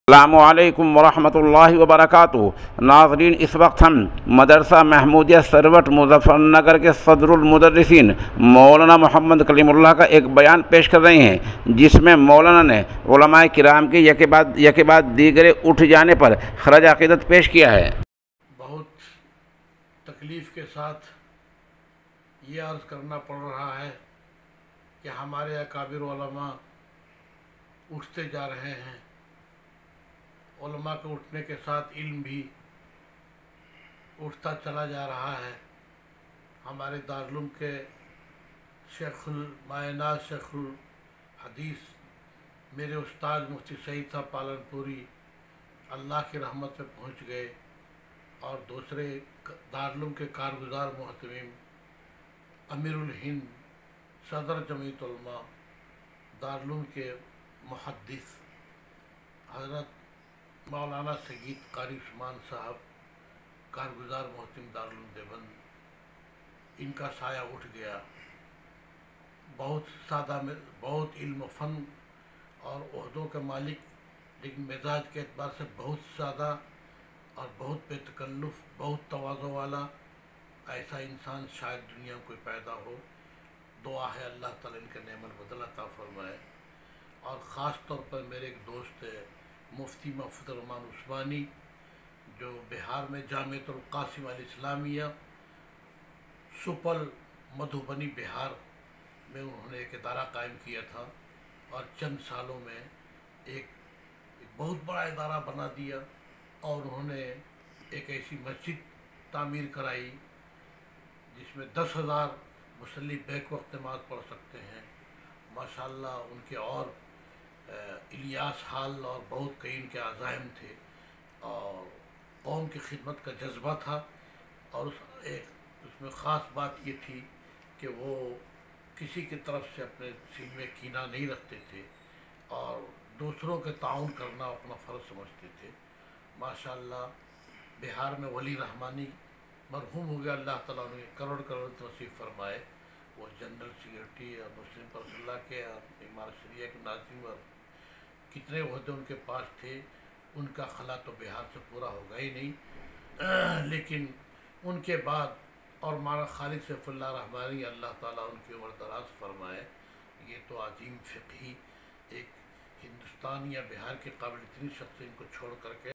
تعزیتی پیغام مولانا کلیم اللہ صدر مدرس مدرسہ محمودیہ سروٹ Maulana kaleem ullah
मौलाना करीमुल्लाह। मदरसा महमूदिया सरवट